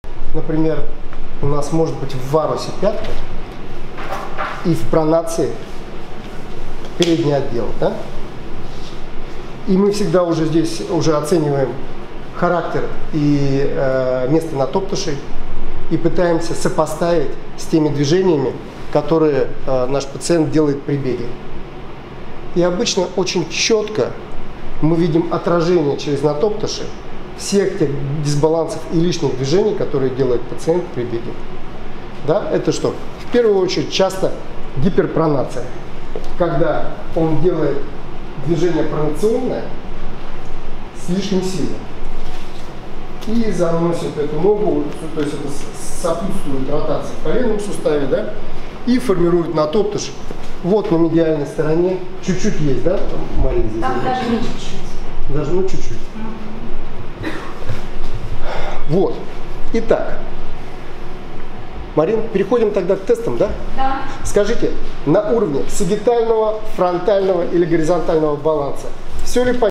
Диагностика стопы на семинаре в Академии Физической Реабилитации, Михеев М.М.(фрагмент семинара).
Максим Михайлович Михеев(АФР) про диагностику стопы для дальнейшего лечения на семинаре. Фрагмент семинара.